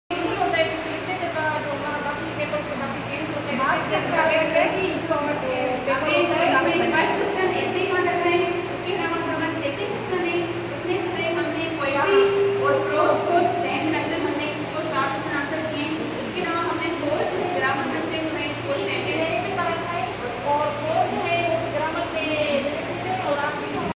Mobile workshop participants presentation
PAF Govt. Girls higher secondary school district Korangi Karachi conducted by DETRC Karachi team